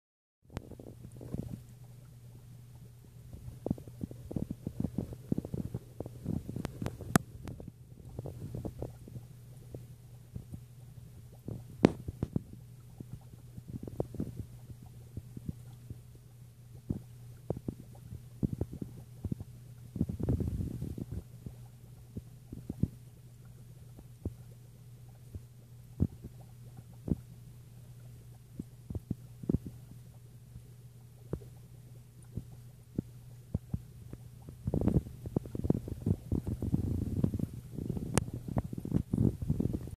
Doryichthys martensii mating?
My new pipefish displaying some new behavior and color, definitely a male/female interaction.